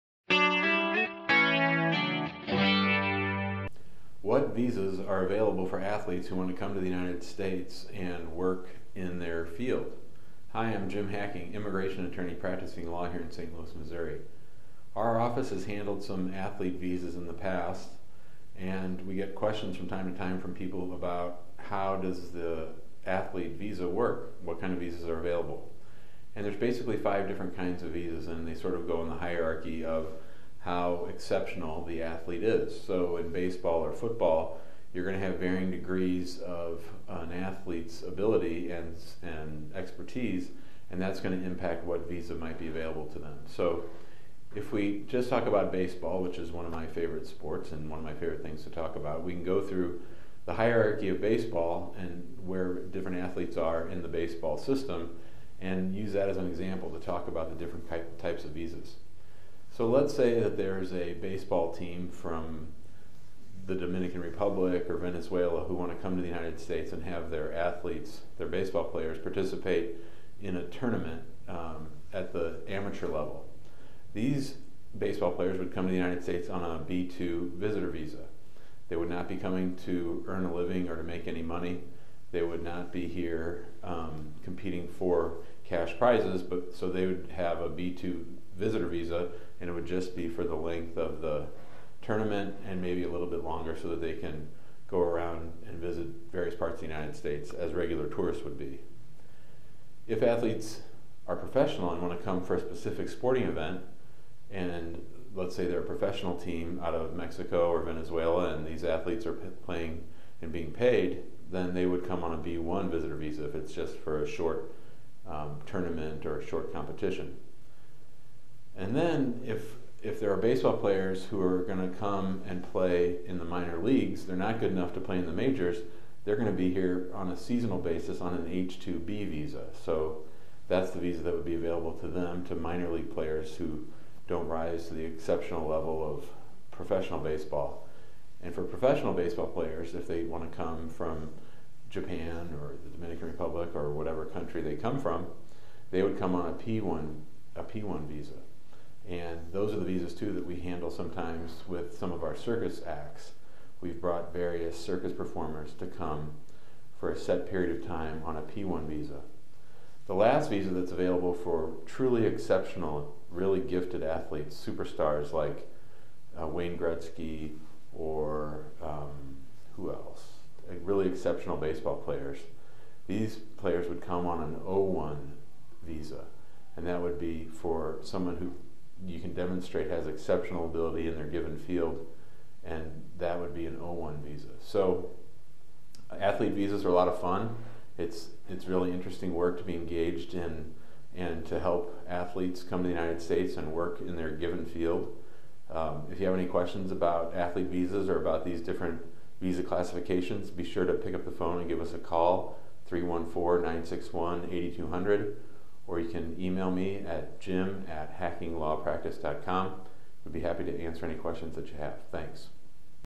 What visas are available for international athletes who want to work or compete in the US?
What visas are available for athletes who want to come to the United States and work in their field? Hi, I'm Jim Hacking, immigration attorney practicing law here in St Louis, Missouri.       Our office has handled some athlete visas in the past, and we get questions from time to time from people about how does the athlete visa work? What kind of visas are available? There's basically 5 different kinds of visas and they sort of go in the hierarchy of how exceptional the athlete is. With baseball or football, you're going to have varying degrees of an athlete's ability and expertise and that's going to impact what visa might be available to them.       If we just talk about baseball, which is one of my favorite sports and one of my favorite things to talk about, we can go through the hierarchy of baseball, and where different athletes are in the baseball system, and use that as an example to talk about the different types of visas.       Let's say that there's a baseball team from the Dominican Republic or Venezuela who want to come to the United States and have their baseball players participate in a tournament at the amateur level, these baseball players would come to the United States on a B2 Visitor Visa. They would not be coming to earn a living or to make any money. They would not be here competing for cash prizes, so they would have a B2 Visitor Visa. It would just be for the length of the tournament and maybe a little bit longer, so that they can go around and visit various parts of the United States as regular tourists would be.        If athletes are professional and want to come for a specific sporting event, and let's say they're a professional team out of Mexico or Venezuela and these athletes are playing and being paid, then they would come on a B1 Visitor Visa if it's just for a short tournament or a short competition. Then, if they are baseball players who are going to come and play in the Minor Leagues ... They're not good enough to play in the Majors ... They're going to be here on a seasonal basis, on a H2B Visa. That's the visa that would be available to them, to Minor League players who don't rise to the exceptional level of professional baseball.       For professional baseball players, if they want to come from Japan or the Dominican Republic or whatever country they come from, they would come on a P1 Visa. Those are the visas too that we handle sometimes with some of our circus acts. We've brought various circus performers that come for a set period of time on a P1 Visa.       The last visa that is available for truly exceptional, really gifted athletes, superstars like Wayne Gretsky or ... Who else? Really exceptional baseball players ... These players would come on an O1 Visa. That would be for someone who you can demonstrate has exceptional ability in their given field. That would be an O1 Visa.       Athlete visas are a lot of fun. It's really interesting work to be engaged in and to help athletes come to the United States and work in their given field. If you have any questions about athletes visas or about these different visa classifications, be sure to pick up the phone and give us a call: . Thanks.